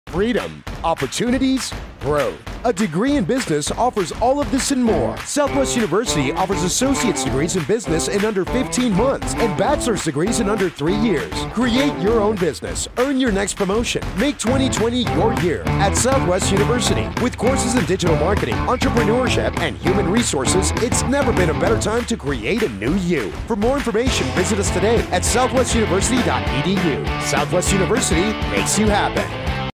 Business 2020 30 sec new
Southwest University offers associate degrees in business in under 15 months and bachelor degrees in under 3 years!

Make 2020 your year at Southwest University